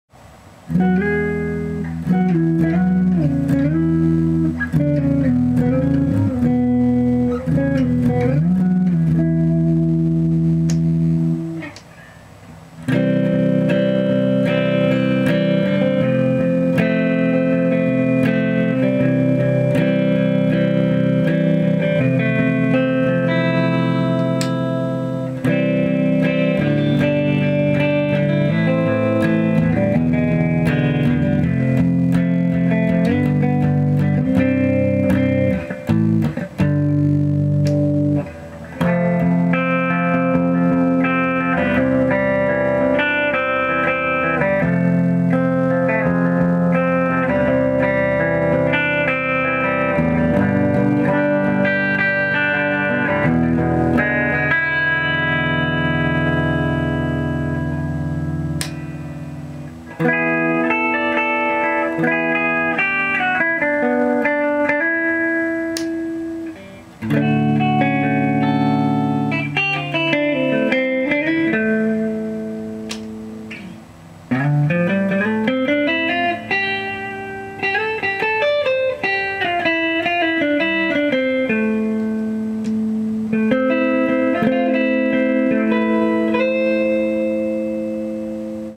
Jaguar HH Demo
Here I showcase some of the sounds of the Jaguar HH through a Twin Reverb. In the future I might record it being used with distortion, as that's where it really shines.